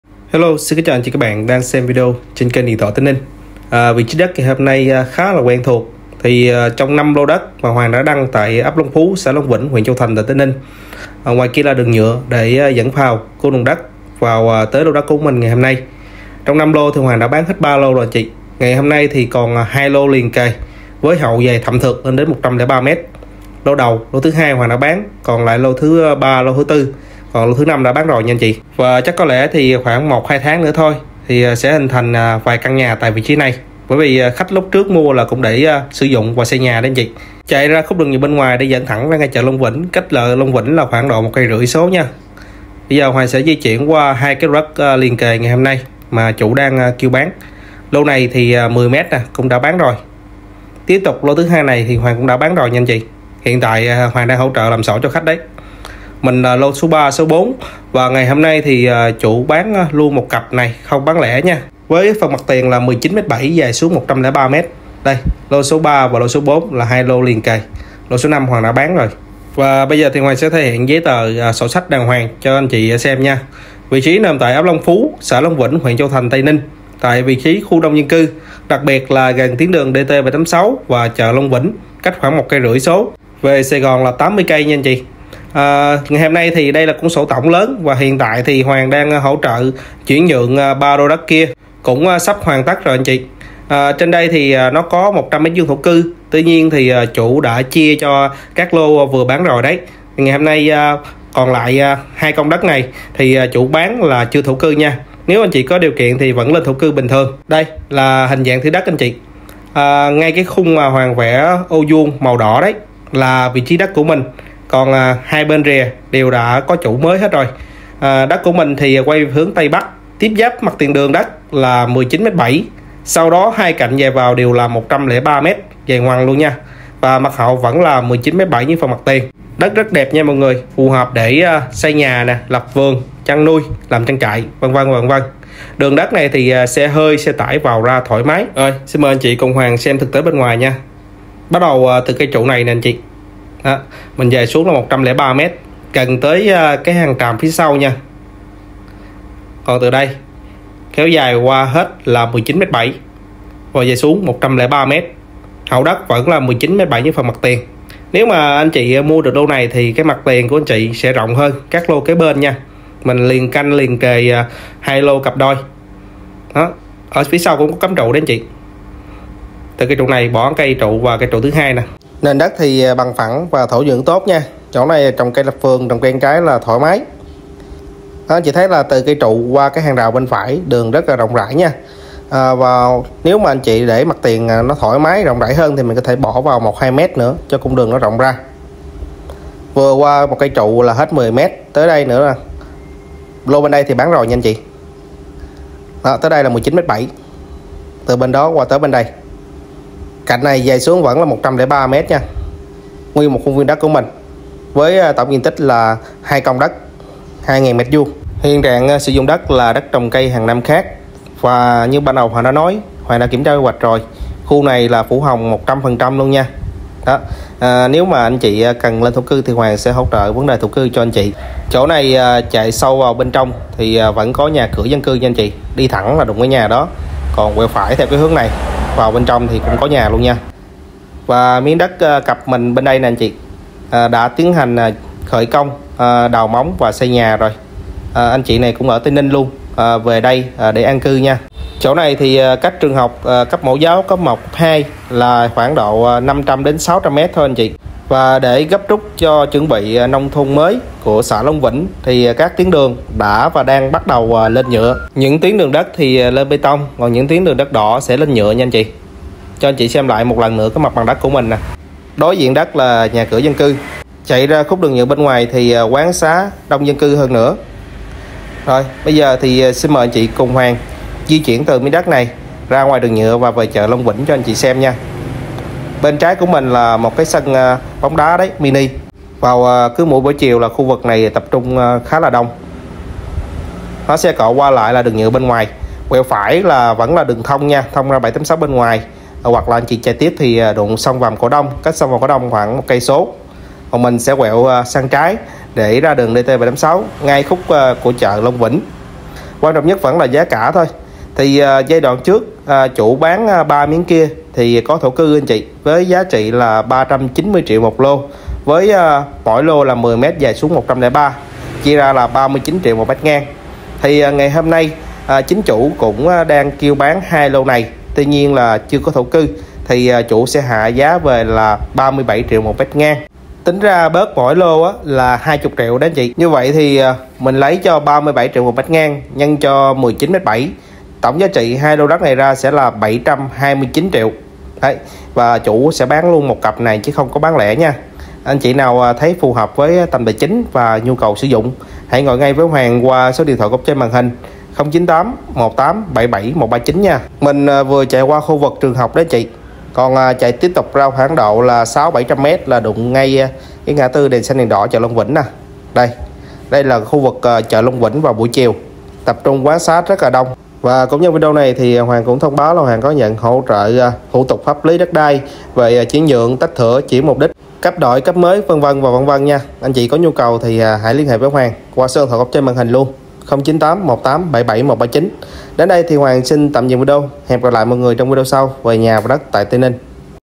Bán Đất Tây Ninh 2000m² Giá Rẻ, Lập Vườn Xây Nhà Thoải Mái, Khu Đông Dân Cư, Gần Chợ Long Vĩnh
🍀 Vị trí: Long Vĩnh, Châu Thành, Tây Ninh.
🌲 Diện tích: 19.7x103m, HNK, lên thổ cư được.
🪴 Hướng: Tây Bắc.
🌳 Tiện ích: Khu đông dân cư, đường oto, xe tải tới đất. Cách chợ Long Vĩnh & đường Đt.786 chỉ 1.5km, cách TP HCM về bến xe An Sương 80km.
💲💲💲 Giá: 37tr/m ngang (giá chốt)

➡️ Để được hưởng quyền lợi ưu tiên từ sản phẩm anh chị đang xem.
▶️ Zalo: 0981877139 (Lưu Thanh Hoàng)